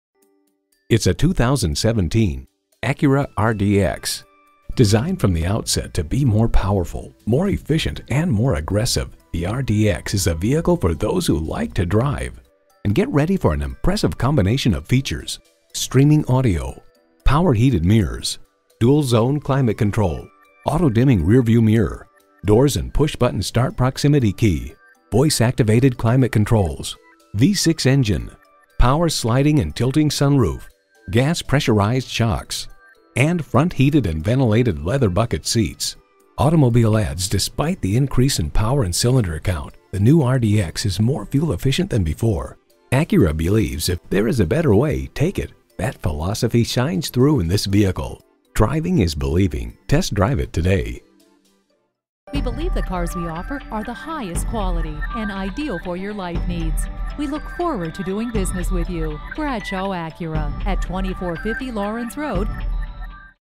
Used 2017 Acura RDX Greenville SC Easley, SC AP5499 - SOLD
Year: 2017
Make: Acura
Model: RDX
Trim: Advance Package
Engine: 3.5L V6 SOHC i-VTEC 24V
Transmission: 6-Speed Automatic
Color: Black
Interior: Parchment
Mileage: 32220
Stock #: AP5499
VIN: 5J8TB3H72HL003195

Address:
2450 Laurens Rd